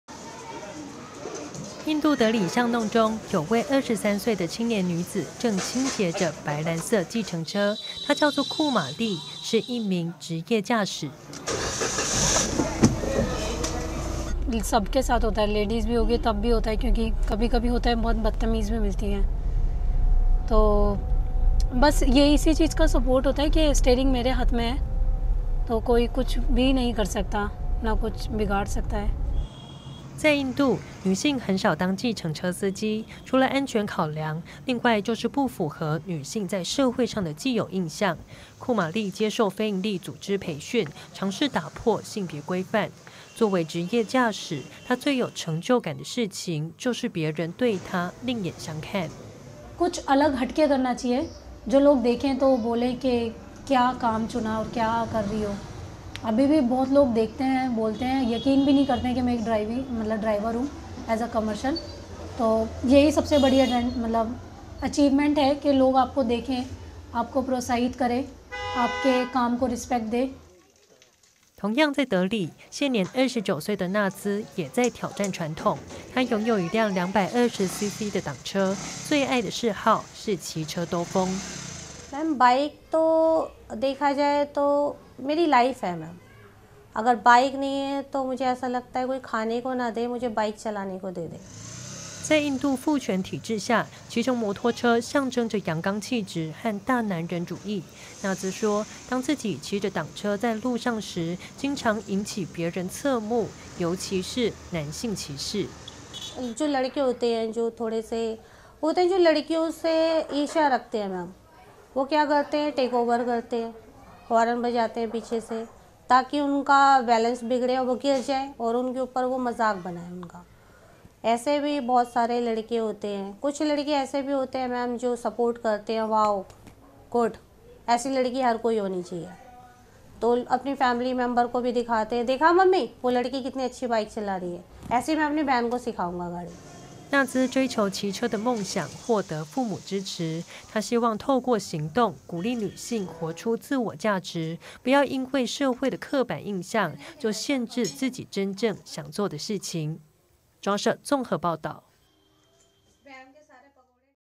印度婦女開計程車騎檔車 盼打破性別框架｜中央社影音新聞
印度在宗教和文化影響下，保有傳統的父權體制，女性很少會騎摩托車或者開車。德里有位婦女納茲就挑戰了傳統規範，她不畏別人異樣眼光騎乘檔車圓夢，鼓勵女性活出自我價值。另外一位青年女子庫瑪莉則成為職業計程車司機，希望自己做的事情能讓人獲得啟發，打破社會上的性別框架。